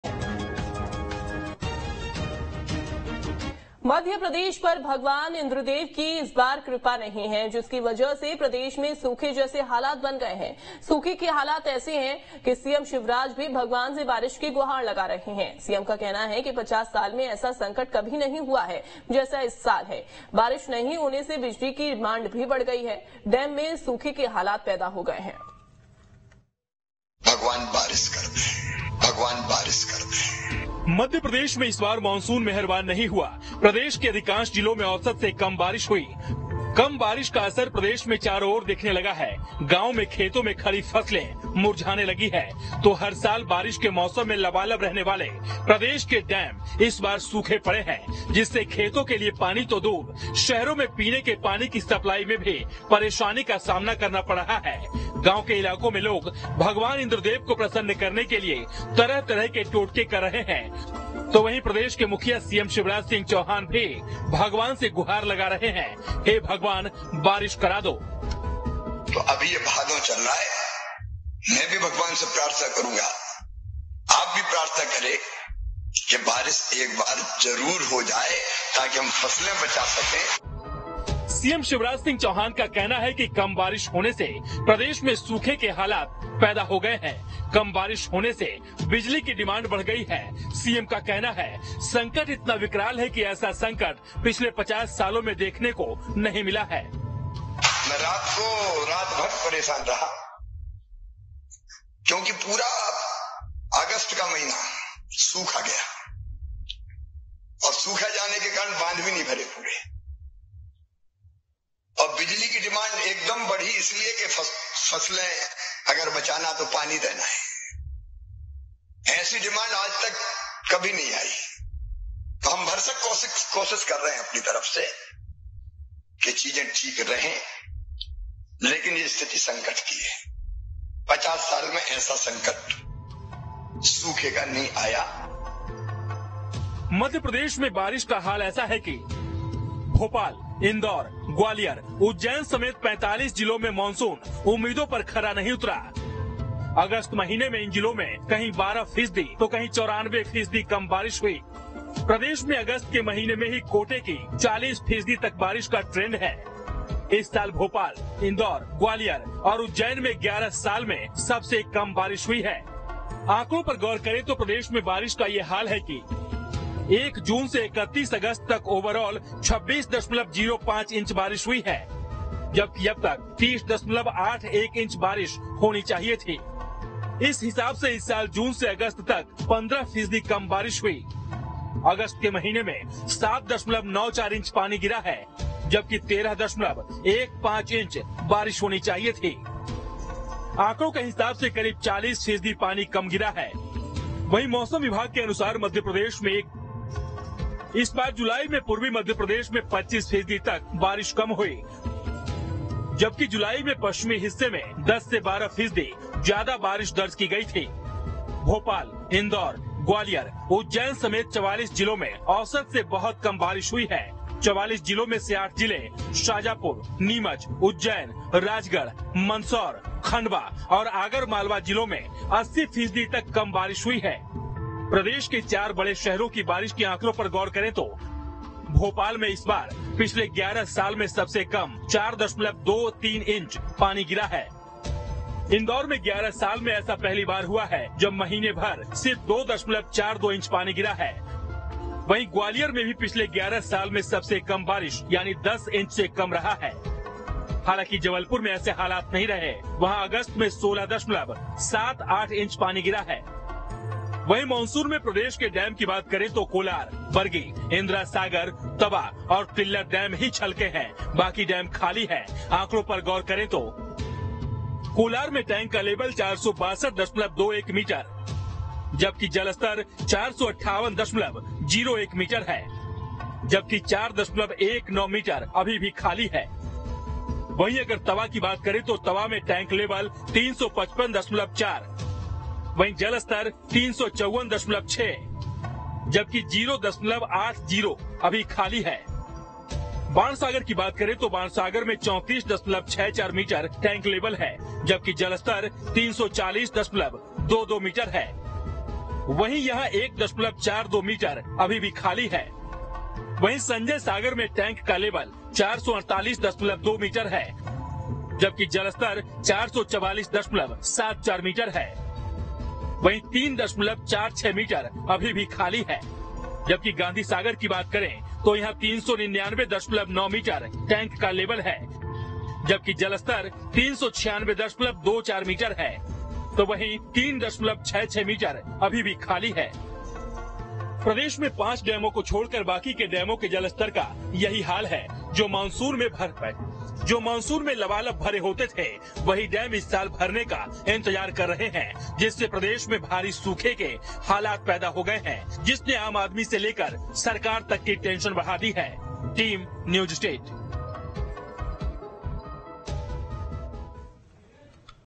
CM Shivraj की भगवान से बारिश की प्रार्थना, 5 डैम छोड़ प्रदेश के सभी डैम हैं खाली | News State MP CG
मध्य प्रदेश पर भगवान इंद्रदेव की इस बार कृपा नहीं है... जिसकी वजह से प्रदेश में सूखे जैसे हालात बन गए हैं... सूखे के हालात ऐसे हैं सीएम शिवराज भी भगवान से बारिश की गुहार लगा रहे हैं... सीएम का कहना है कि 50 साल में ऐसा संकट कभी नहीं हुआ है... जैसा इस साल है... बारिश नहीं होने से बिजली की डिमांड बढ़ गई है... डैम में सूखे के हालात पैदा हो गए हैं... देखिए रिपोर्ट...

This time Lord Indradev has not blessed Madhya Pradesh... due to which drought like situation has arisen in the state... The situation of drought is like this, CM Shivraj is also praying to God for rain... CM says That such a crisis has never happened in 50 years... like it is this year... the demand for electricity has increased due to lack of rain... drought conditions have arisen in the dam... see the report...